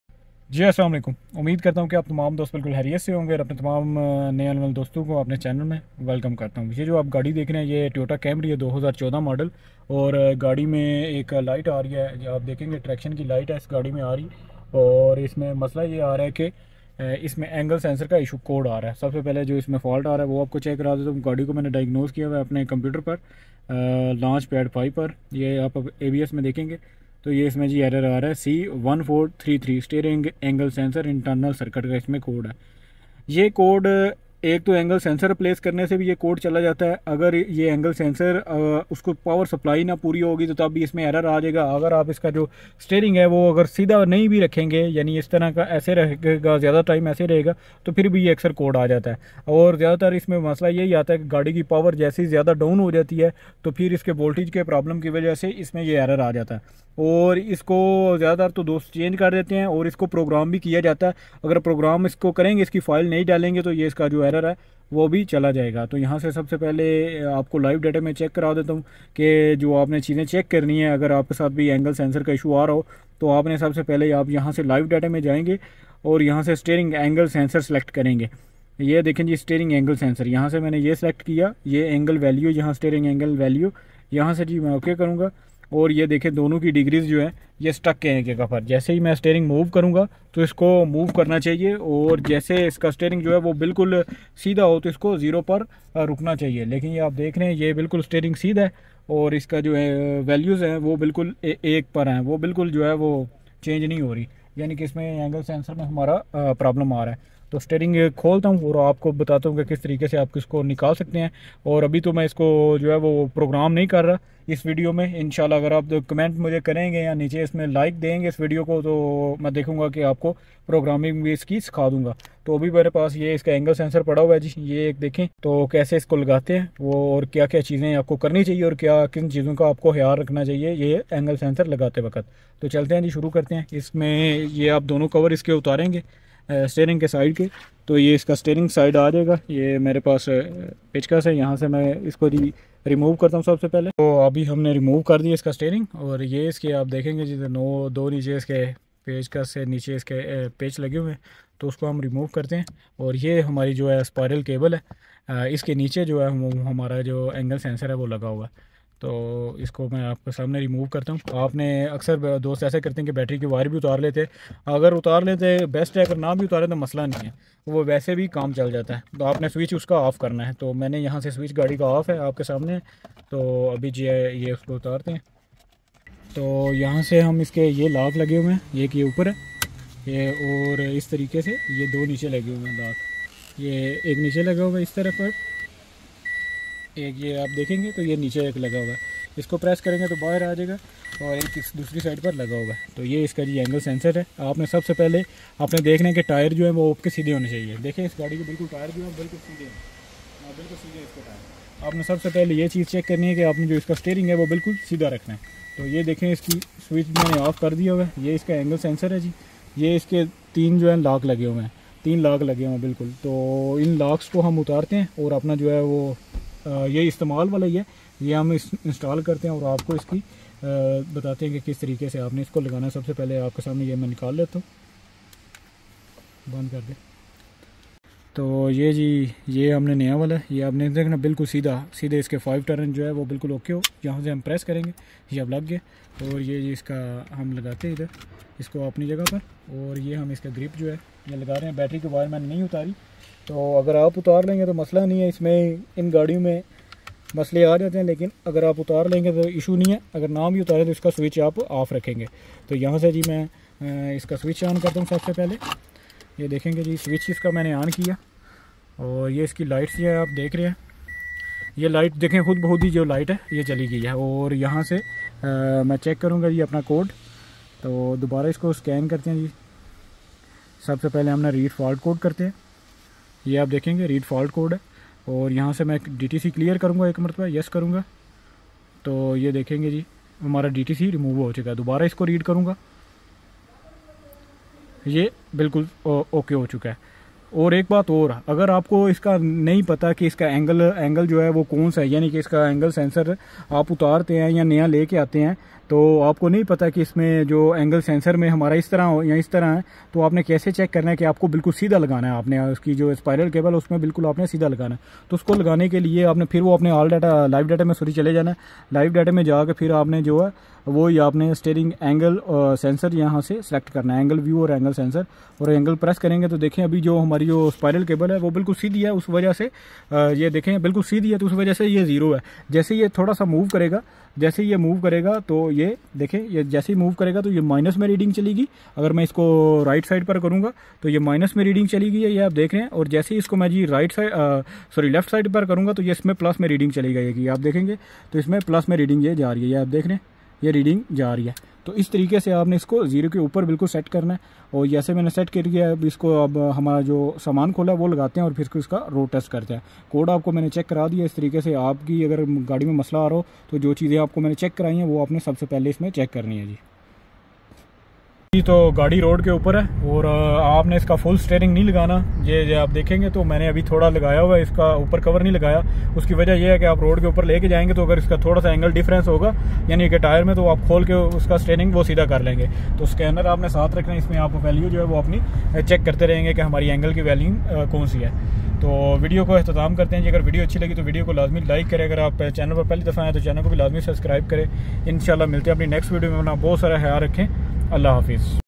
c1433 steering angle sensor internal circuit || Solutions with complete guide 💯
Hi Frindes I hope are fine in this video I show you how to fix c1433 steering angle sensor internal circuit in toyota camry/ toyota raw 4 with complete guide and solution